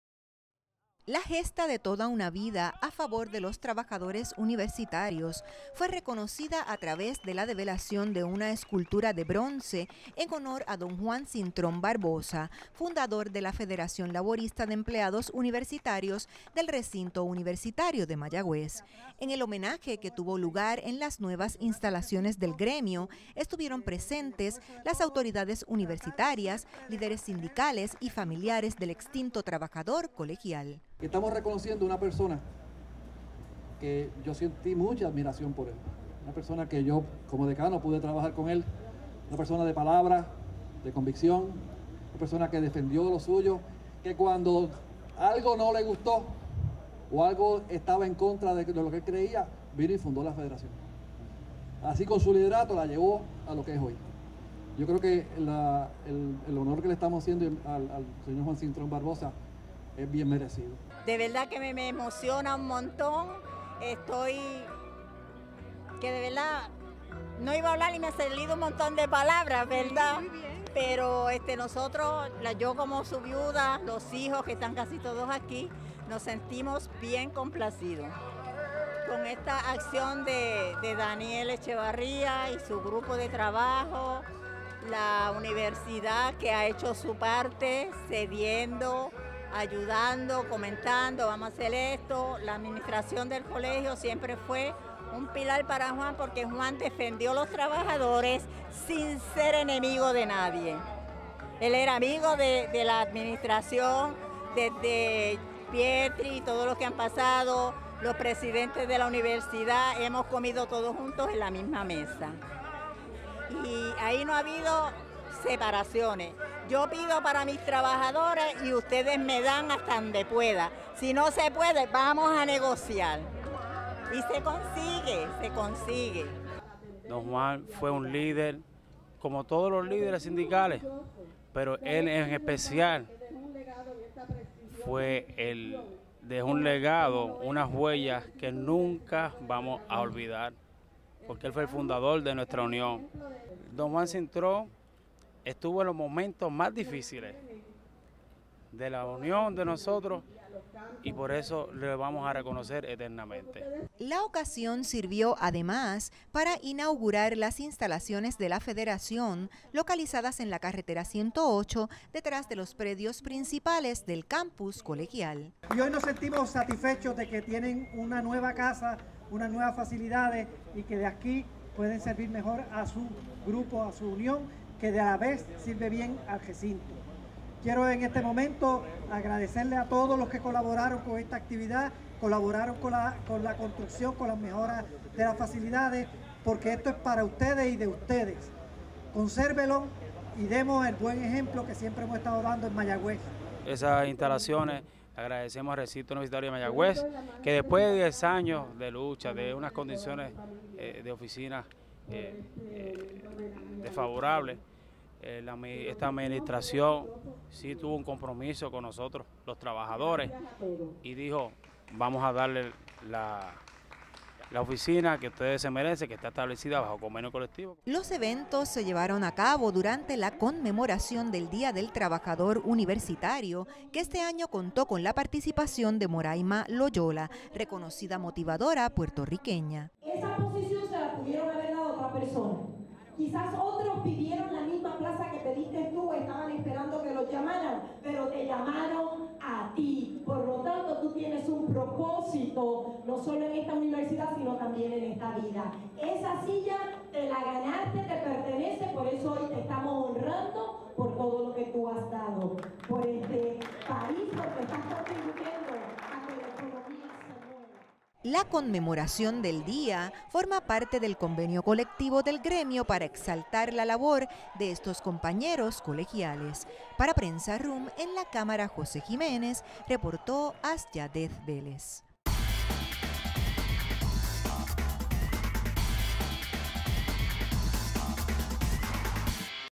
Develan escultura en honor al fundador de FLEURUM
La gesta de toda una vida, a favor de los trabajadores universitarios colegiales, fue reconocida a través de la develación de una escultura en bronce en honor a don Juan Cintrón Barbosa, fundador de la Federación Laborista de Empleados Universitarios del Recinto Universitario de Mayagüez (FLEURUM). En la cámara, José Jiménez Orfila; reporta Azyadeth Vélez Candelario. Videorreportaje producido por Prensa RUM.